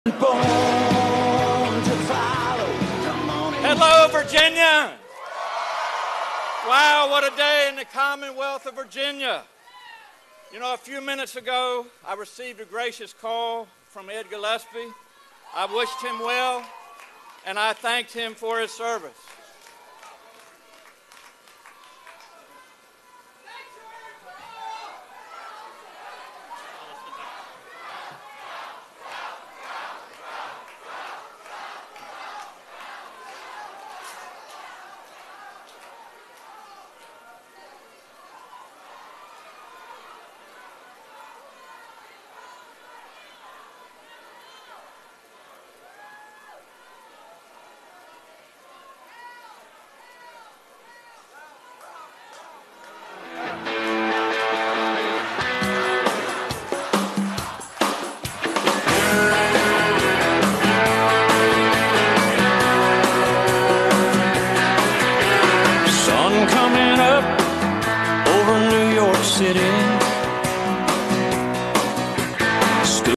Dems Divided: Liberals Heckle Ralph Northam At His Own Election Night Party, Forcing Him Off Stage
Dems Divided: Liberals Heckle Ralph Northam At His Own Election Night Party, Forcing Him Off Stage (November 7, 2017)